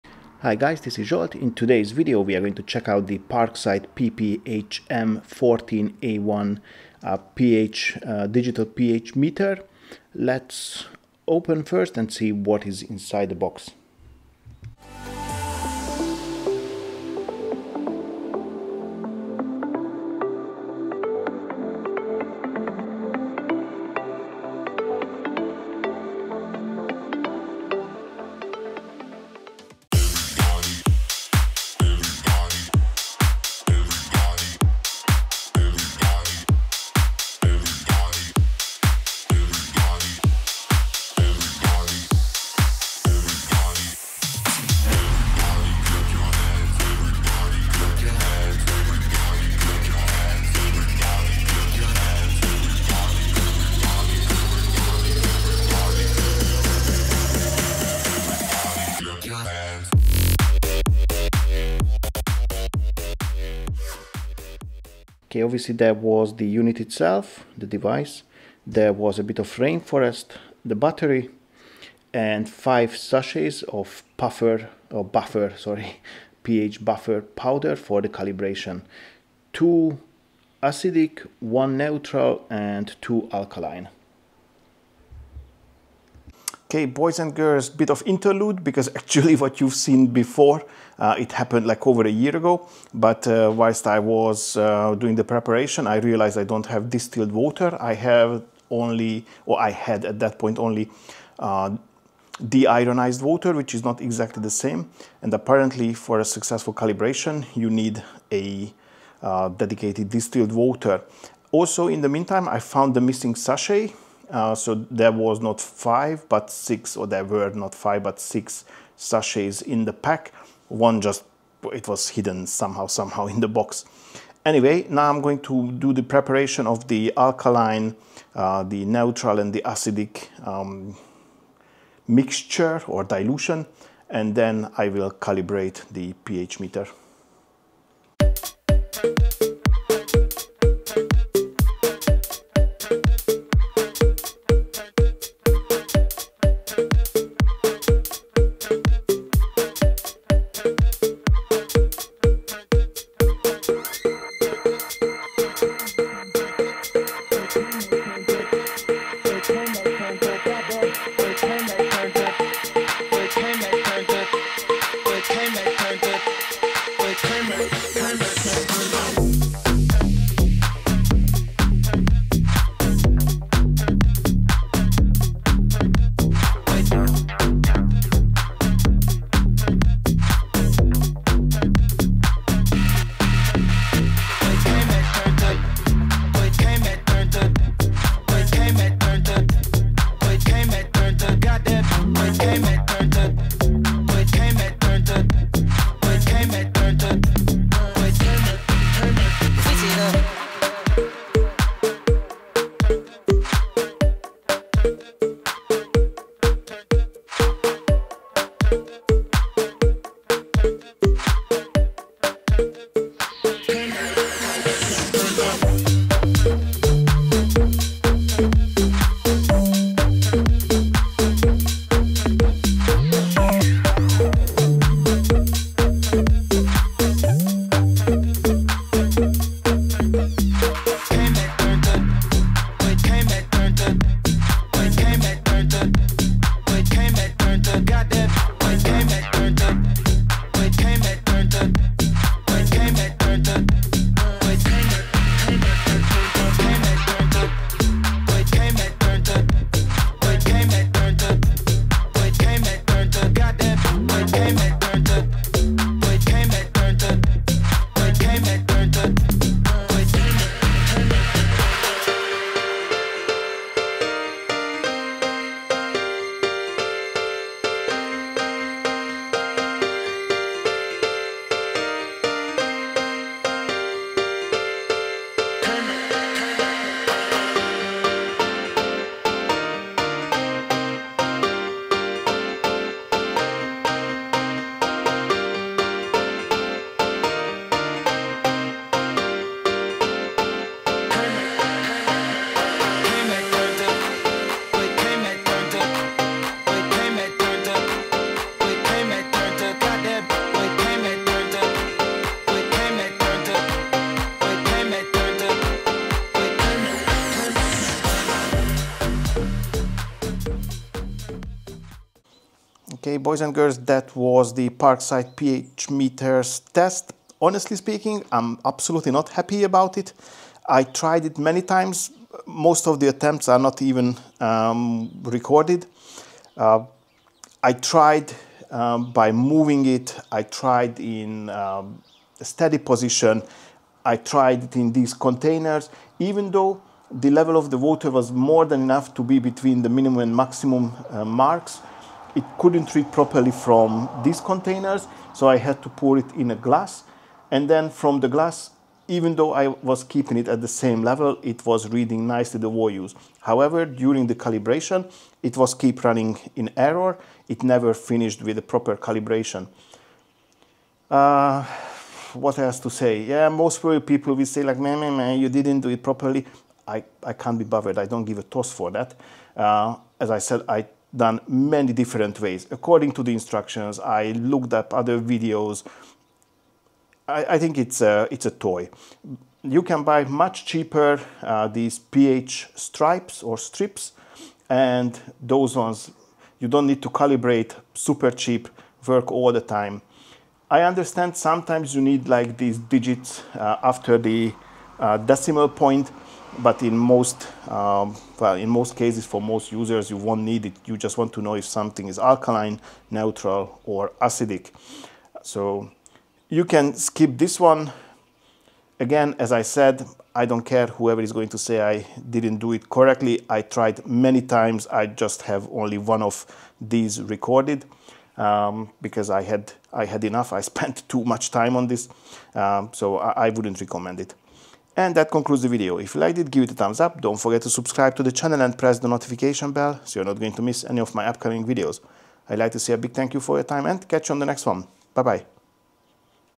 Parkside PPHM 14 A1 (digital pH meter) test - EN
This is a non-sponsored video of the Parkside PPHM 14 A1 (digital pH meter)'s test.

Sonax car care products: sonax.hu